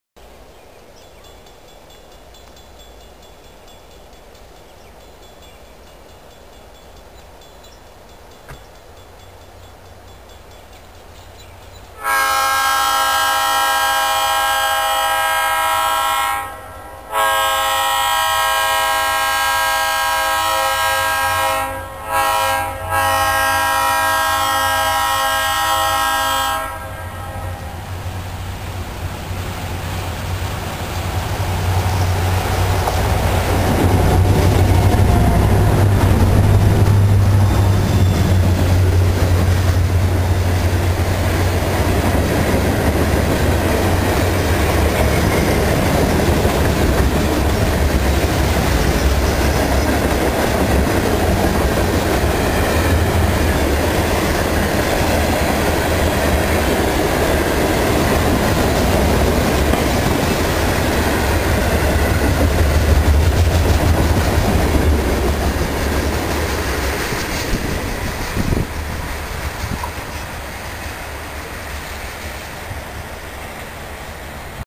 B&LE SD38's through the dusk at Springboro Pa.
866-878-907 lead a loaded southbound through at 8:55PM!! May 21st 2009